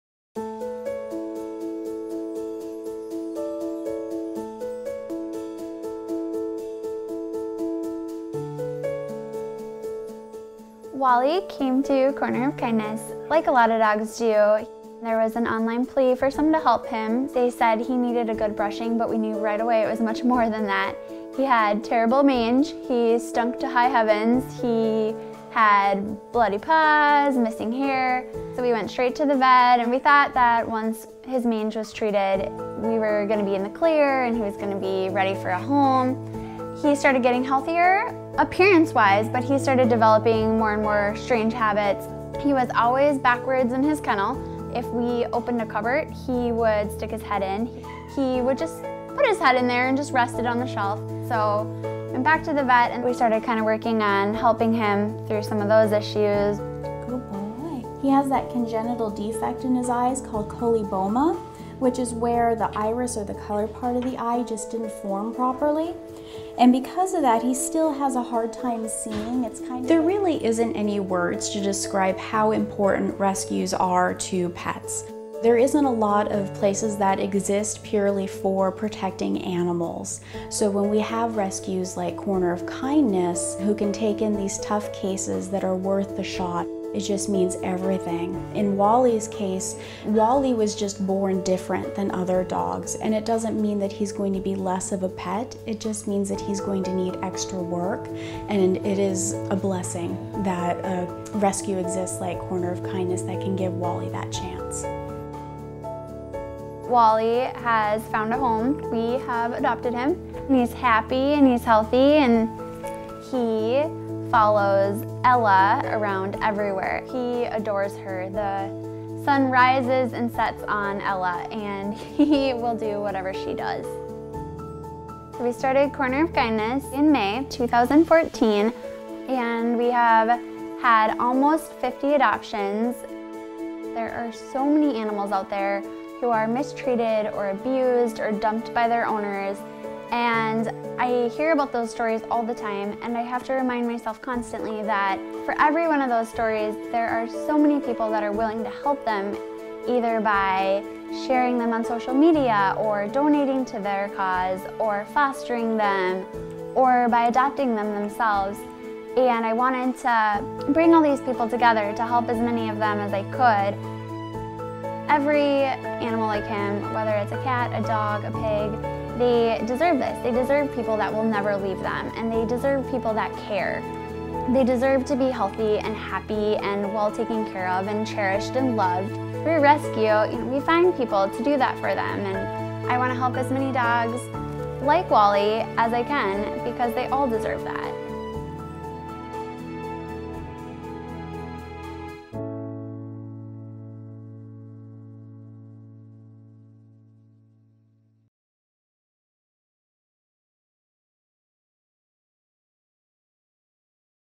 Corner of Kindness
Produced by Enke Films
Corner of Kindness animal rescue helps healthy animals as well as sick, scared, and hospice animals. This video features just one of those many special cases- an australian shepherd named "Wally the Wolf" who has a congenital defect that has caused him to have severely limited vision, lessened hearing, and the pace at which he can learn is much, much slower than average dogs. In addition, he had mange when he first came to rescue and has many digestive issues that are carefully monitored and managed so he can continue living his happy lifestyle! Wally has been adopted, his story is meant to inspire everyone to help those in need and to spread the word of what real animal rescues do and why they are so important; so more dogs like Wally can be rescued and adopted into the loving families they so deserve.